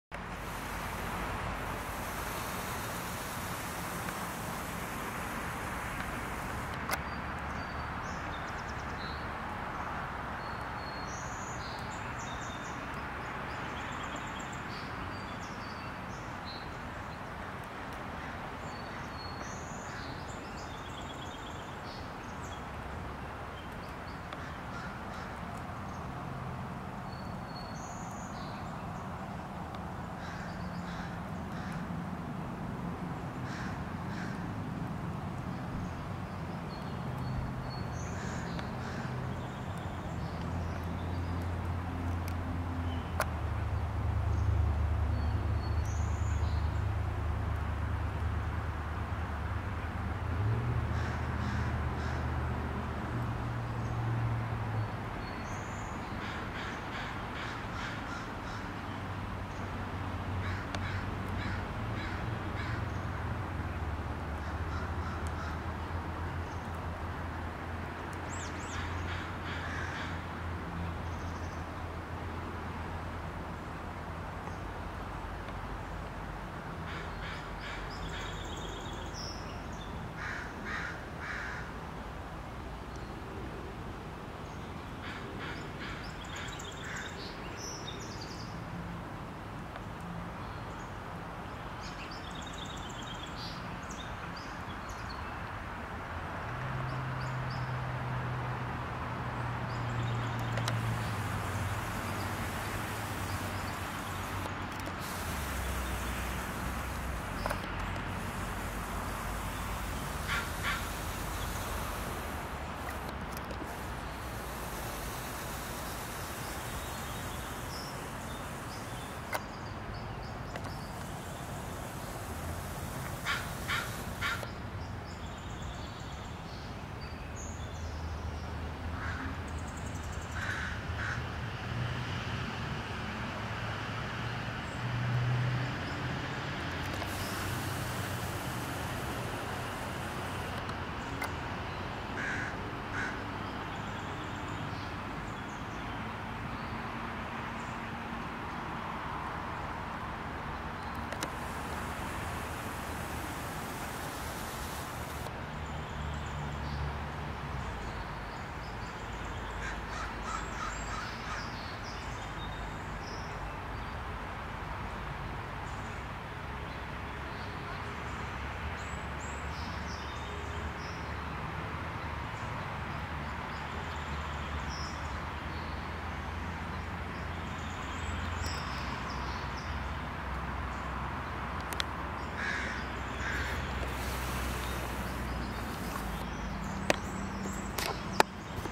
Douglas squirrel (Tamiasciurus douglasii)
The Douglas squirrel (Tamiasciurus douglasii) is a pine squirrel found in the Pacific coastal states and provinces of North America.